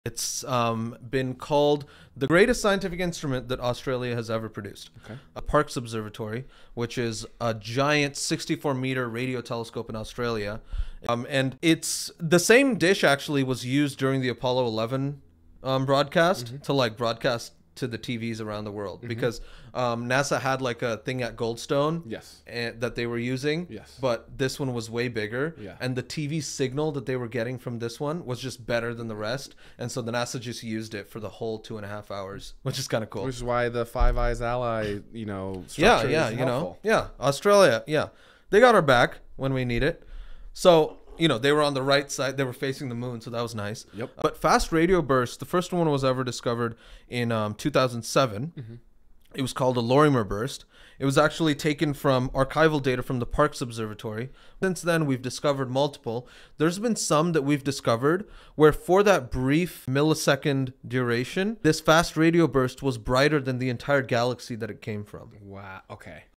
Brightest Radio Burst EVER: Is it Aliens or Cosmic Mystery?
The Parkes Observatory, a giant radio telescope in Australia, was used during the Apollo 11 broadcast. NASA used this telescope because it provided a better TV signal than the rest.